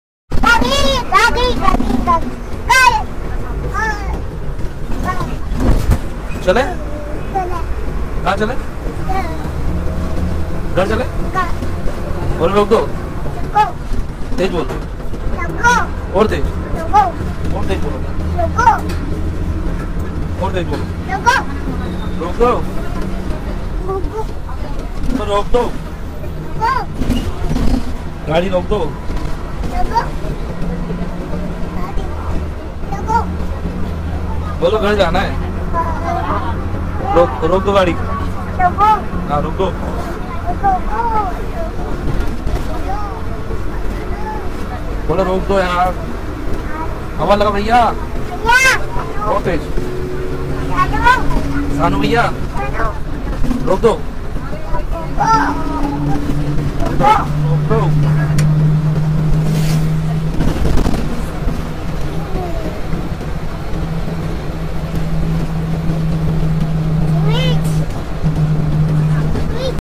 गुड़िया ने बोला गाड़ी रोको 😂😂
Achanak Gudiya Rani Ne Kaha gadi Roko last Tak Dekhe Gadi Ki rukayi

shanu bhai ki yaad aayi to bol diya gadi roko